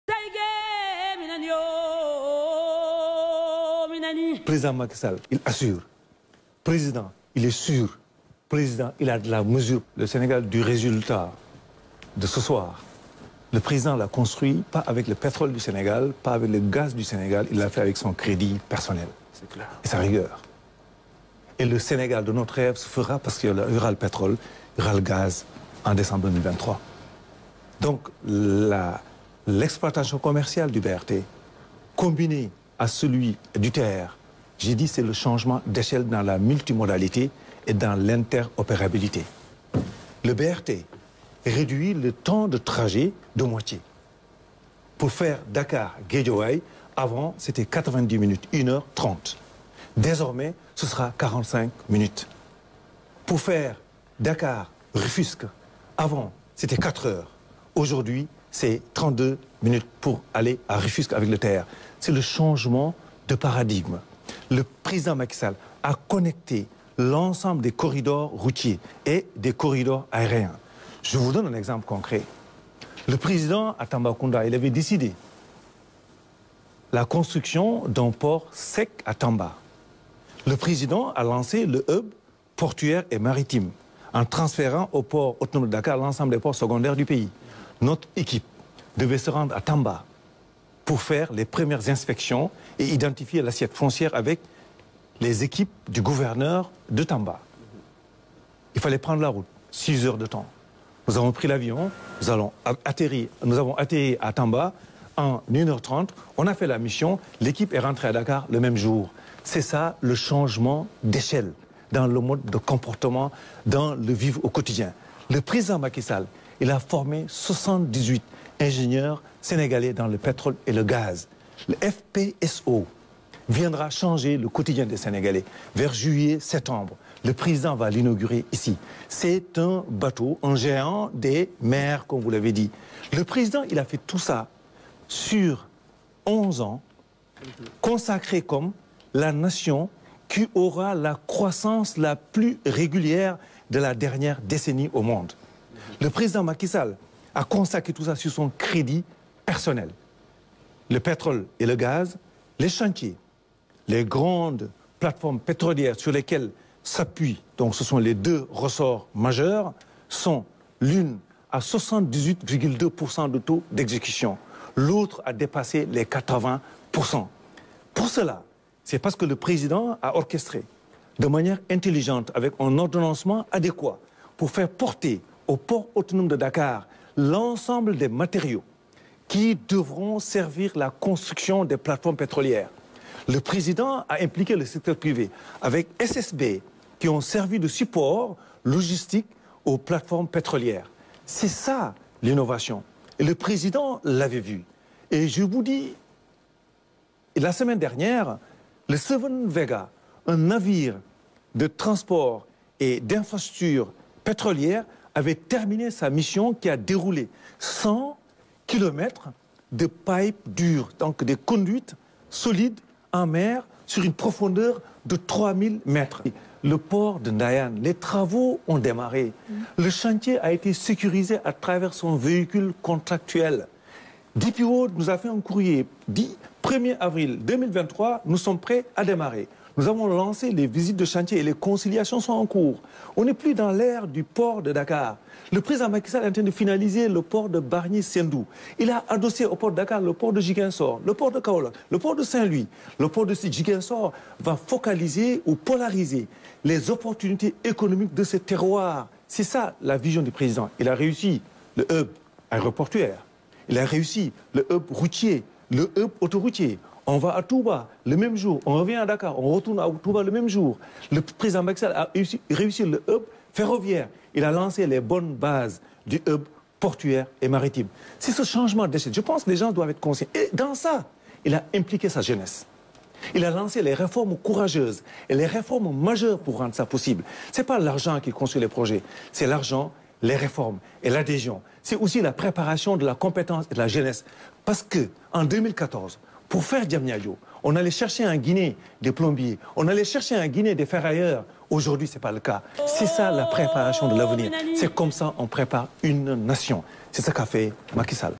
Mountaga SY: "Le Sénégal du résultat de ce soir, c’est le crédit personnel de Macky SALL »
Selon Mountaga Sy, le Directeur Général du PAD: "Le Président Macky SALL assure, rassure, Macky SALL est sûr et il a de la mesure. Le Sénégal du résultat de ce soir, le Président Macky Sall l'a construit avec son crédit personnel et sa rigueur. Le Sénégal de nos reves se fera avec le pétrole et le gaz". Le Maire de Aere Lao était l'invité du plateau décryptage de l'adresse à la Nation de Macky Sall sur la RTS

7TV est une chaîne généraliste qui exploite
 avec un accent particulier sur l'information.